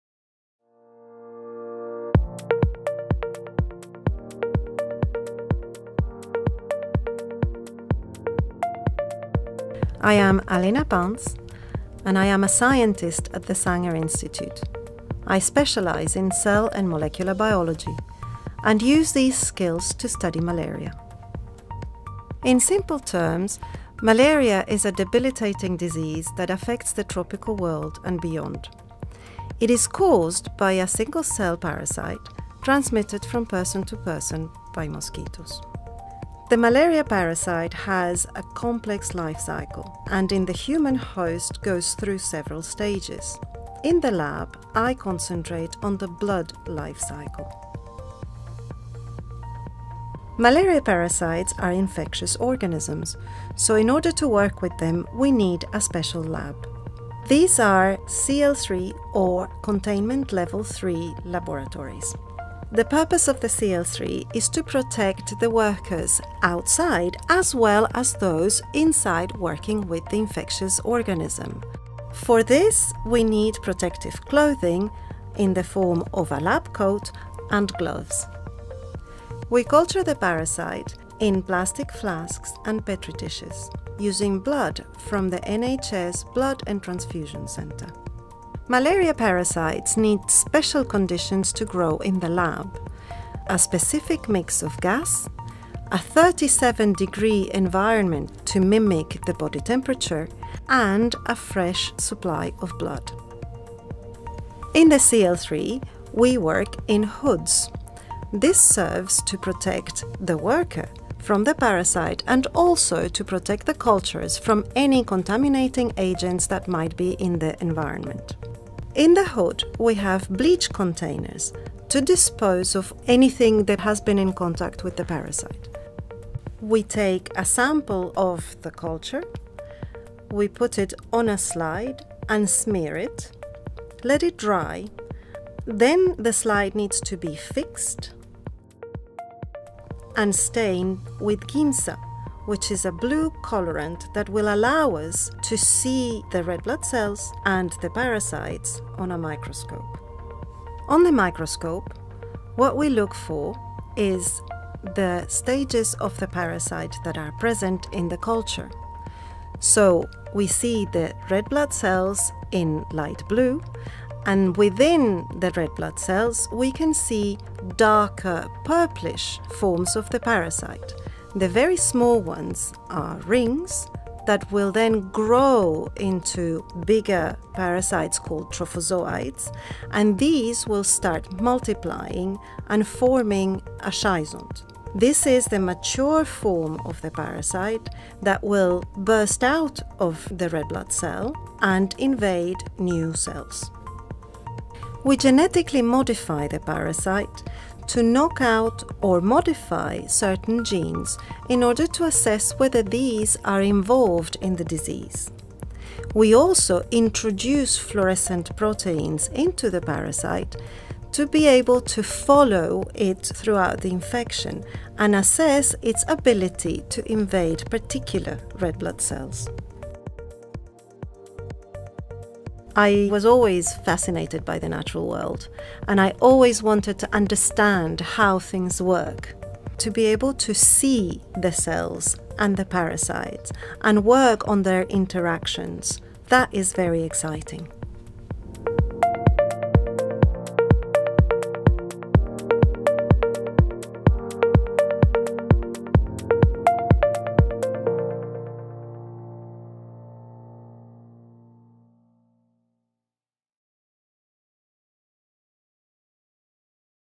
Life in the Lab Working with malaria parasites (Must Watch!)
We delve deep into how malaria parasites work here is an in depth study in this video and the process involved within these studies.